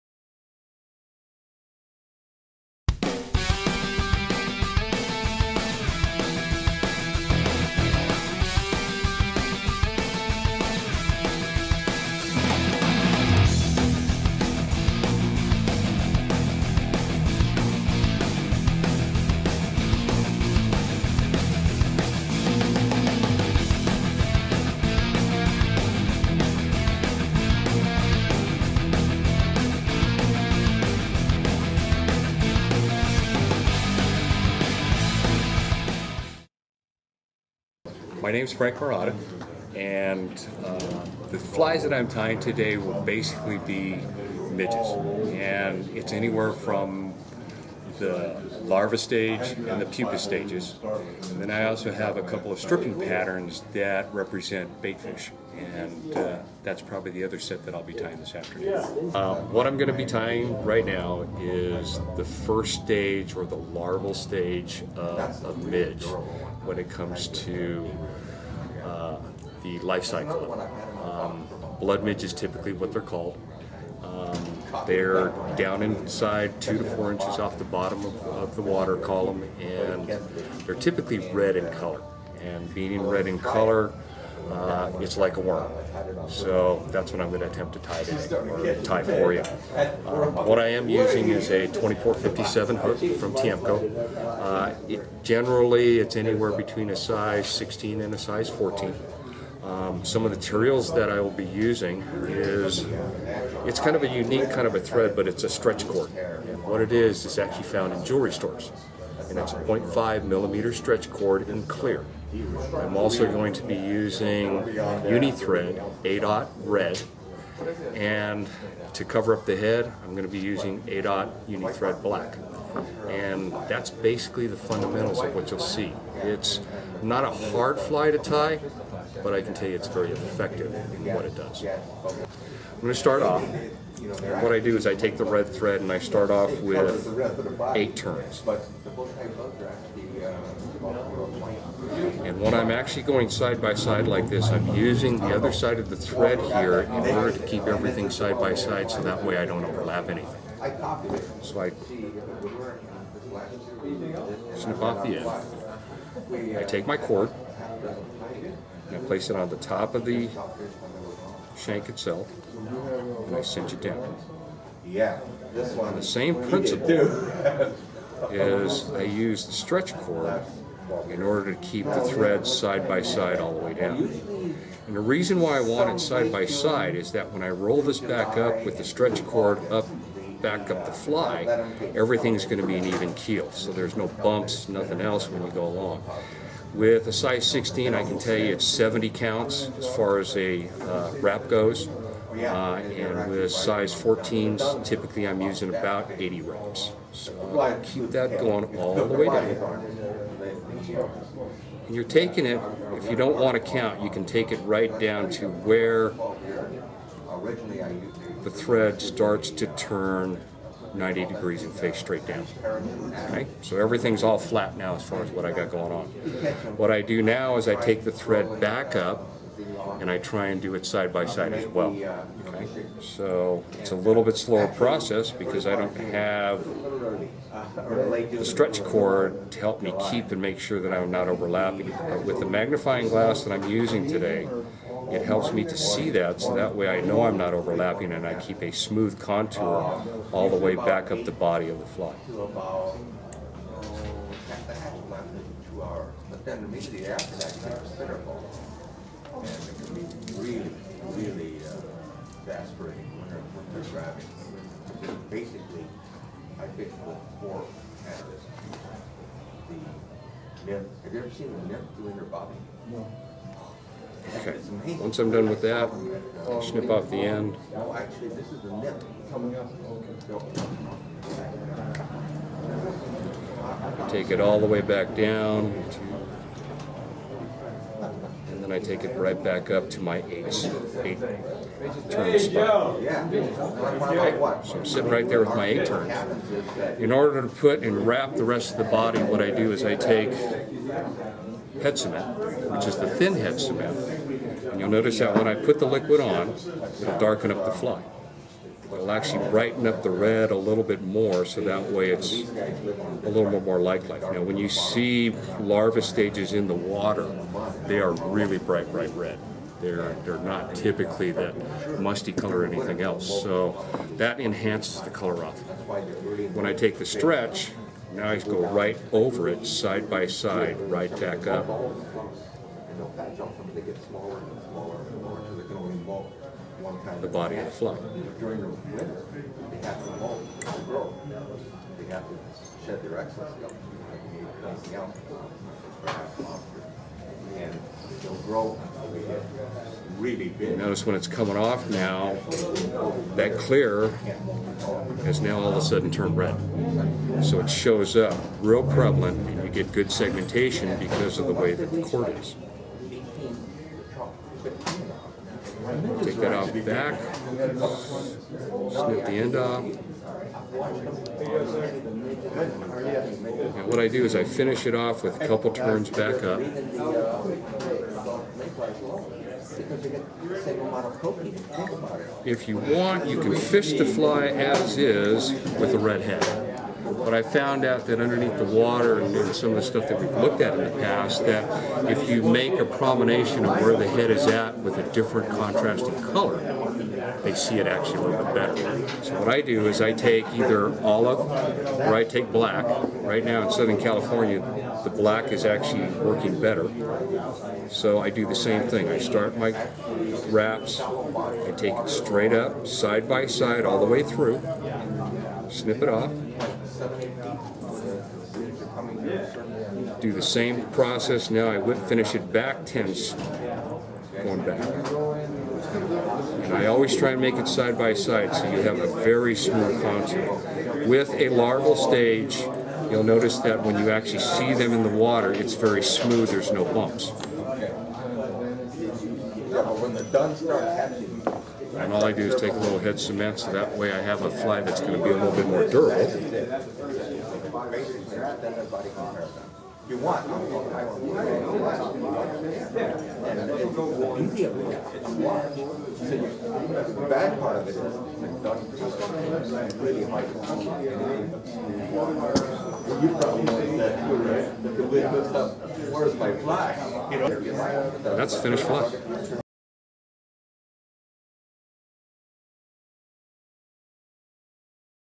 Frank Murata Ties the Blood Midge.mov
Frank Murata, flyfishing pro and winner of several Stillwater Fishing competitions, ties a fly representing the pupal stage of a Chironomid.  This is a deadly pattern in Eastern Sierra waters such as Lake Crowley, and even in Southern California freshwater destinations such as Lake Laguna Niguel.